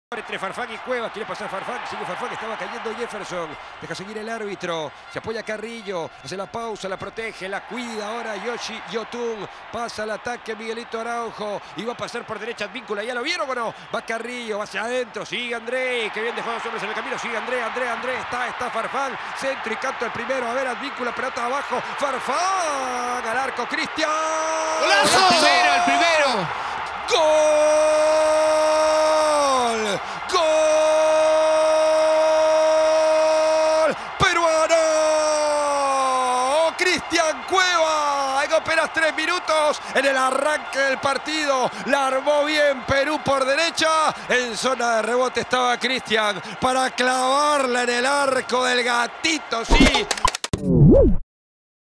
Perú vs. Paraguay: el GOLAZO de Christian Cueva que anotó para la Selección Peruana
El '8' de la SelecciónPeruana anotó un verdadero golazo con un remate cruzado que fue imposible de atajar para el portero de Paraguay. Mira el análisis táctico de la anotación.